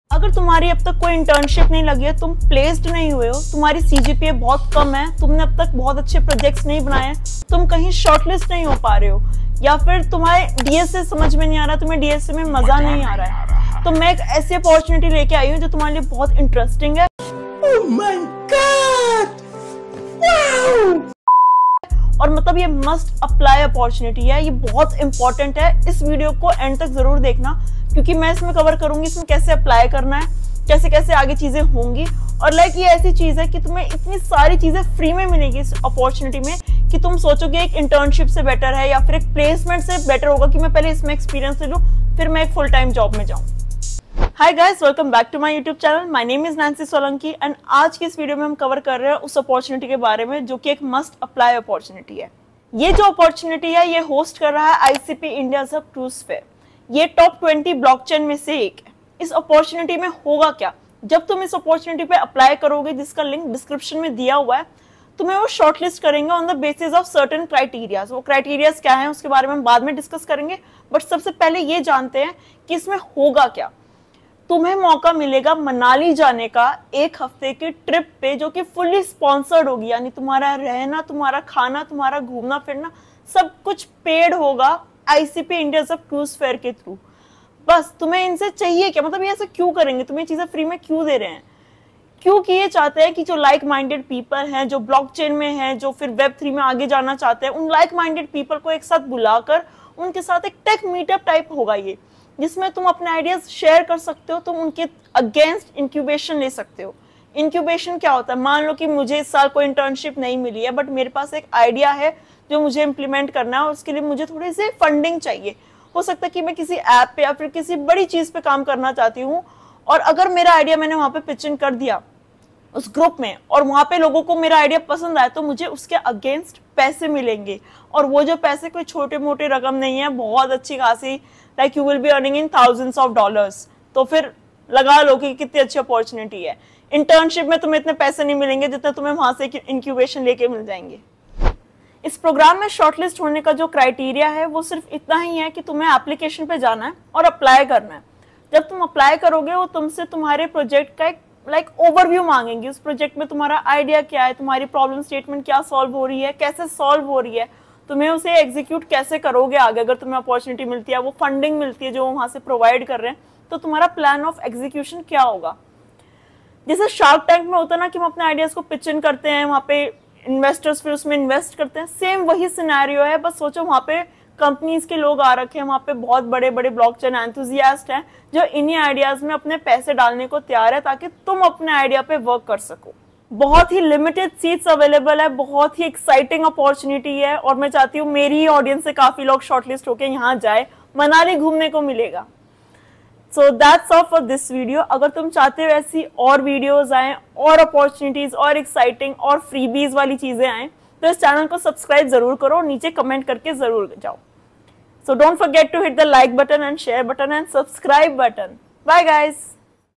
Earn Lakhs & Chance to Visit Manali FREE | Apply Now
In this video, Learn about ICP India’s Hub Crewsphere and their groundbreaking initiatives, including hackathons, incubator programs, and partnerships, all aimed at bridging the gap between Web 2.0 and the decentralized future of Web3.

Get mentorship and funding against your idea‼️